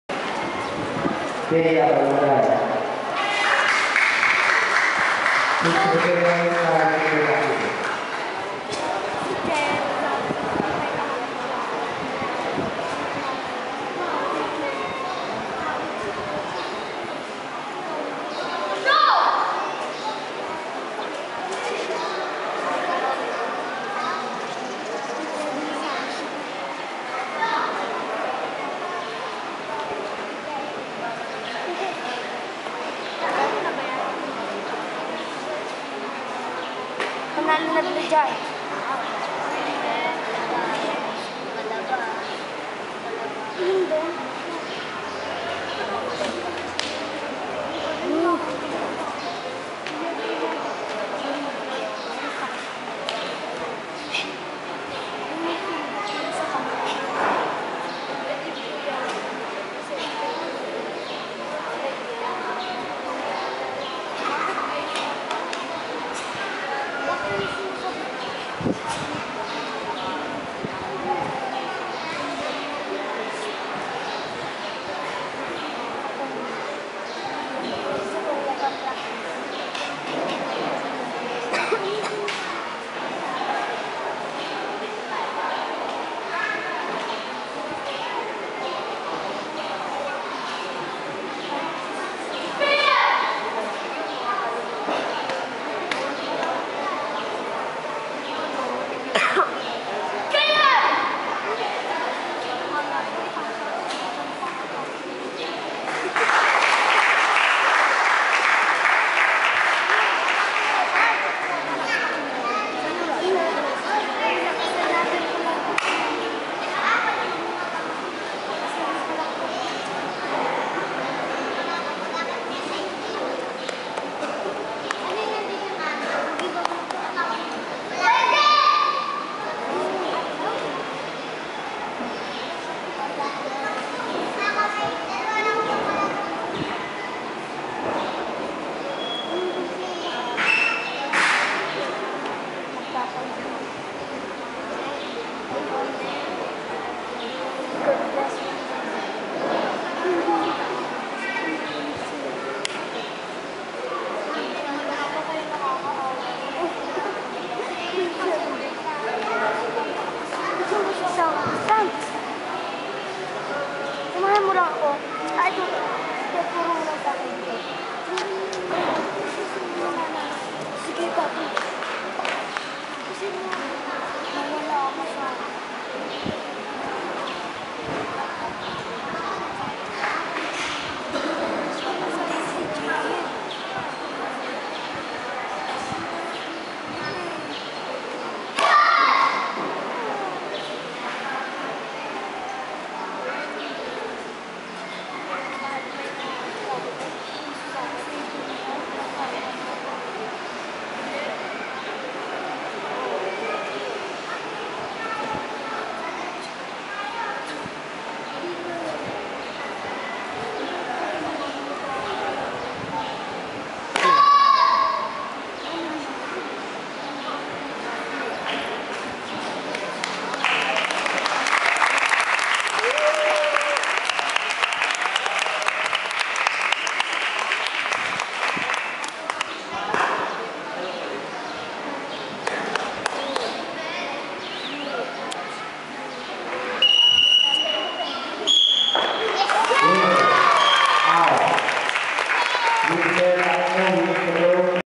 BEA BALUNGAYA - Silver Medalist
Dave's Cup 2010 held at Sta. Rosa Educational Institute Sta. Rosa Laguna last April 10, 2010.  Category - Advance Kata 11years old below.